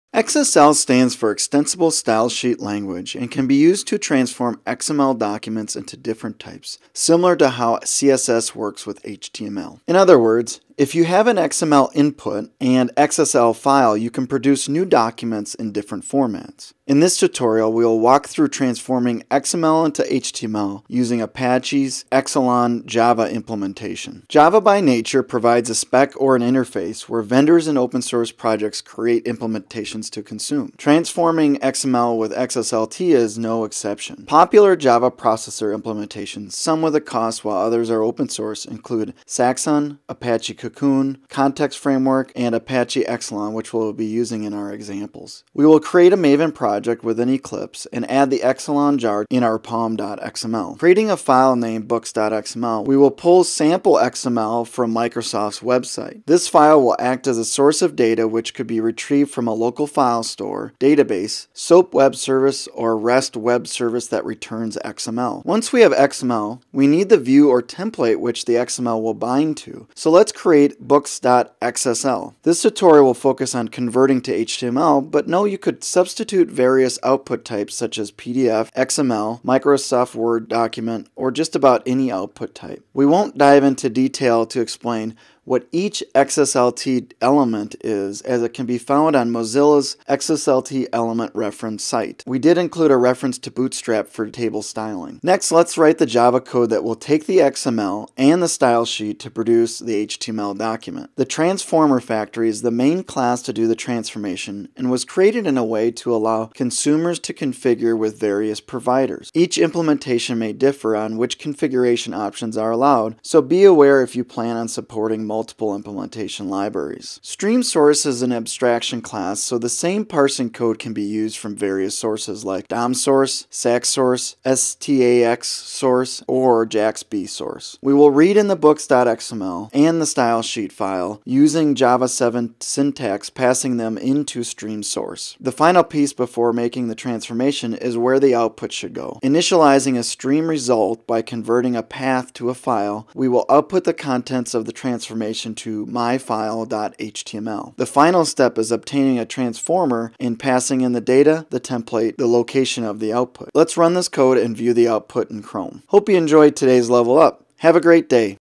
Screencast #20: Tranform xml into html using XSLT XALAN
XSLT provides a way to transform a XML document into various format types. In this screencast find out how to convert XML into HTML using a java based XSLT transformer.